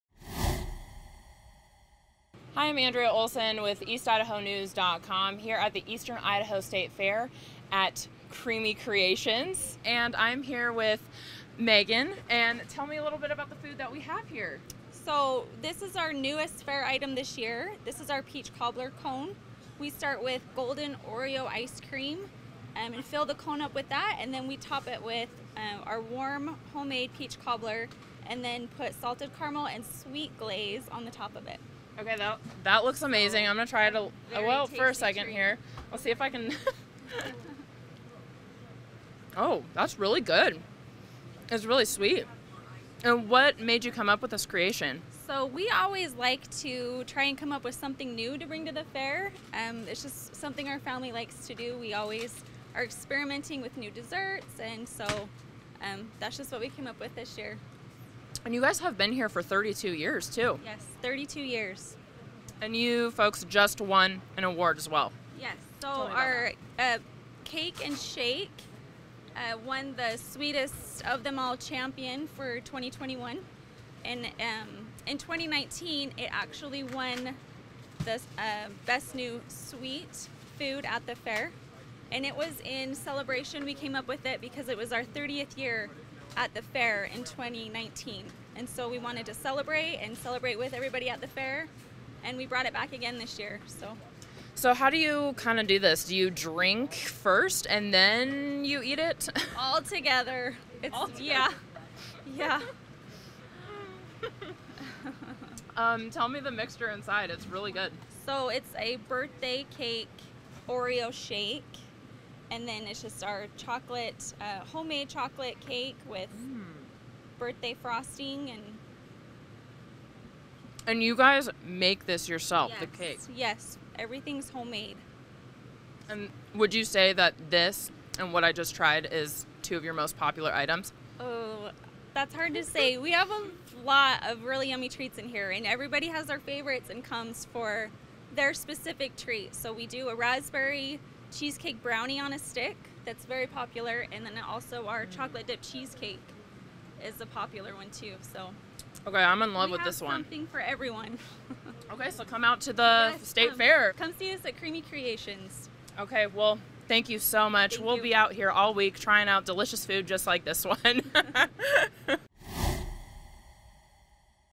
East Idaho Eats: Creamy Creations serves up sweet concoctions at the fair
BLACKFOOT — Creamy Creations has been serving up sweet concoctions at the Eastern Idaho State Fair for 32 years.

One of the newest items on their menu is the peach cobbler cone, which has golden Oreo ice cream inside a waffle cone. It’s then topped with warm peach cobbler and has a salted caramel and sweet glaze on top of it.

“We always like to come up with something new for the fair. We always are experimenting with new desserts and so that’s just what we came up with this year,” said Megan Heward with Creamy Creations.

This year, Creamy Creations won an award for their Birthday Cake Shake & Bake in the Best Fair Food Competition. They took first place in the Best Sweets Champion 2021 category.

“In 2019, it actually won best new sweet food at the fair and it was in celebration we came up with it because it was our 30th year at the fair in 2019 and so we wanted to celebrate with everybody at the fair and we brought it back again this year,” she said.

The birthday cake Oreo shake is topped with homemade chocolate cake and birthday frosting with sprinkles.

“We really have a lot of yummy treats in here and everybody has their favorites and comes for their specific treat. We do a raspberry cheesecake brownie on a stick that’s very popular and then our chocolate-dipped cheesecake is a popular one. We have something for everyone!” said Heward.

The Eastern Idaho State Fair runs through Sept. 11.